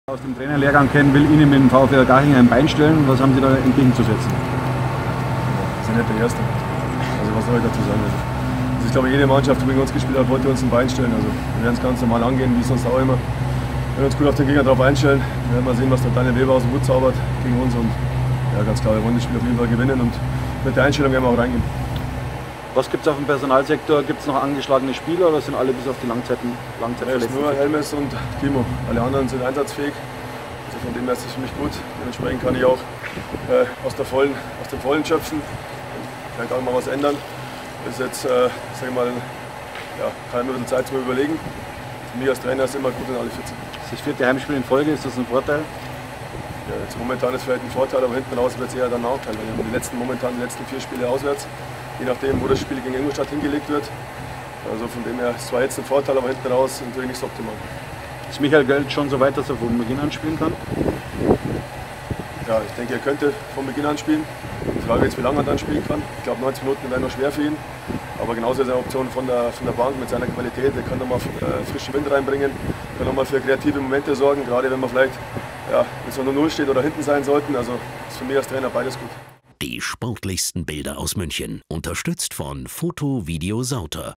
Die Löwen-PK vor dem Spiel gegen VfR Garching
Kann der Trainer bei dem Spiel aus den Vollen schöpfen oder fallen wichtige Spieler aus?